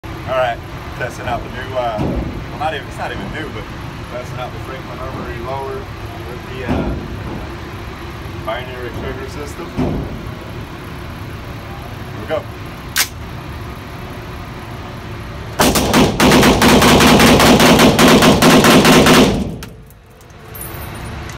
Franklin armory binary trigger system shoots 30 rounds in less than 3 seconds
Firepower